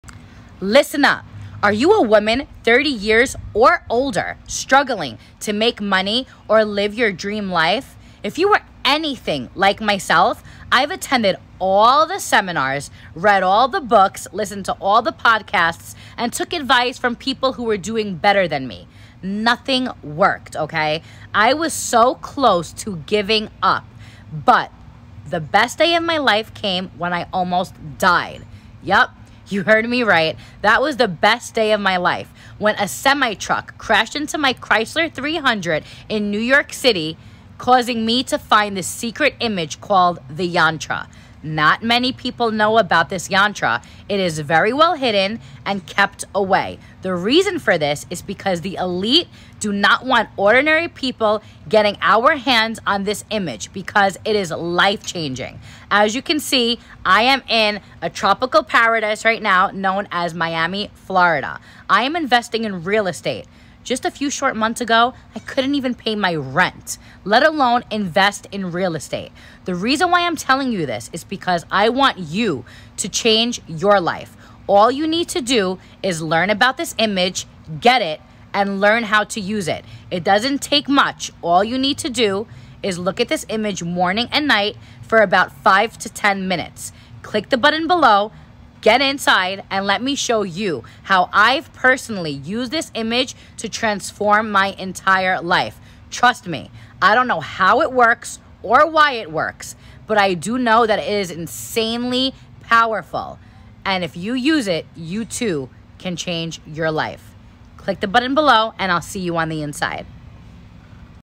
Listen up! Are you a woman 30 years or older struggling to make money or live your dream life ?
In this video, It revealed that

Ancient Manifesting Yantra Manifests Wealth In Just 600 Seconds

Do you really know how to meditate correctly and manifest faster whatever you want in your life?
Maybe you have already meditated for a while and have you ever thought why it still doesn't work on you? why it's not you? why? why? why?

If you'd like to discover more "real secret of the secrets"...then you should go check for more info!